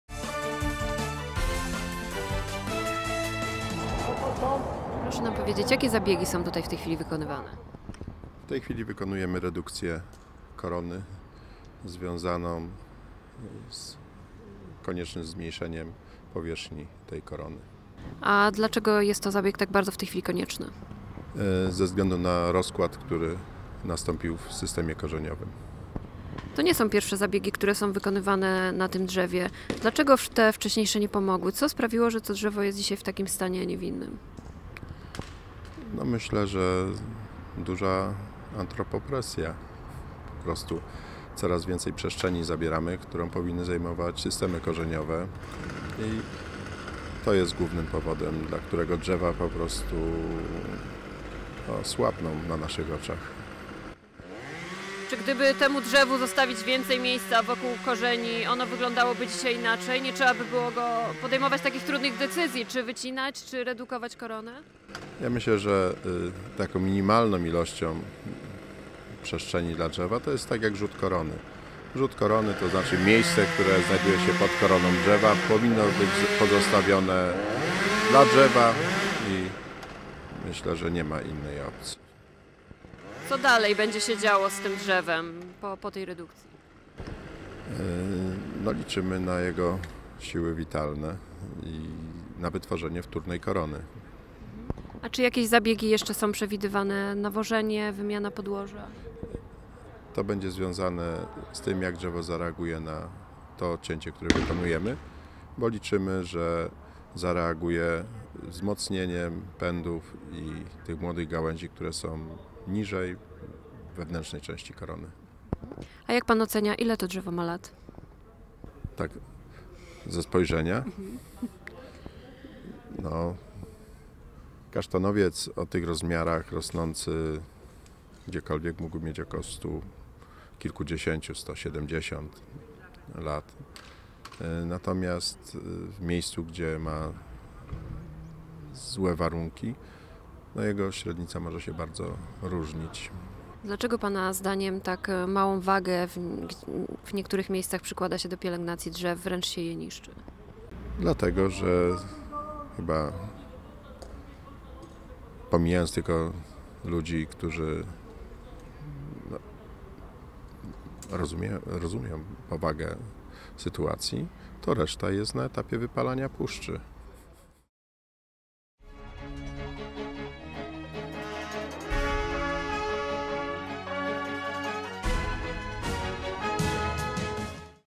Ratują kasztanowca!
- Liczymy na to, że redukcja korony pomoże temu drzewu przetrwać. Jego siły witalne są ogromne, co widać po młodych zieleniących się pędach, ale korona jest zbyt duża, a miejsca dla korzeni zbyt mało. Jeśli nic nie zrobimy, drzewo może się złamać, a nawet przewrócić - mówi Jerzy Stolarczyk z firmy EKO-TREK z Wrocławia, który dzisiaj przeprowadził kolejne prace pielęgnacyjne na kasztanowcu przy świdnickiej katedrze. Korona drzewa została zredukowana o ok. 70 %.